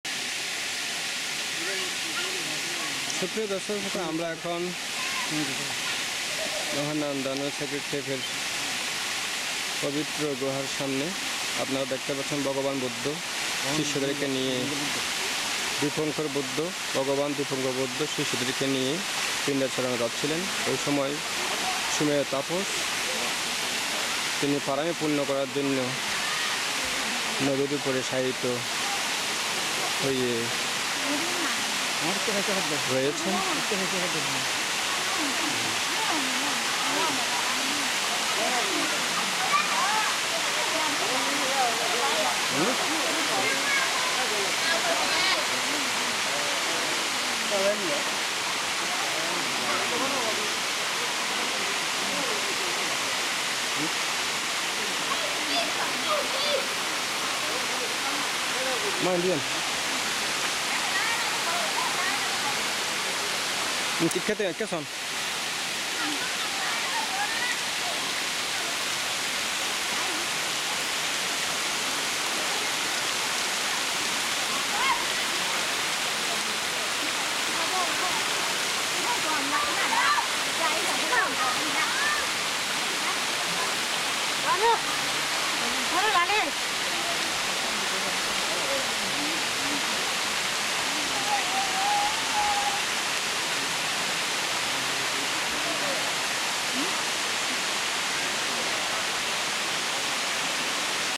Ven. Saccananda and his mom visited at Mahanandanu Sacred Cave in Meimuo city. Part 02
Ven. Tejavatta Bhante, Ven. Ariyakitthi Bhante, Ven. Saccananda and his mom visited at Mahanandanu Sacred Cave in Meimuo city in Myanmar on January 02, 2015.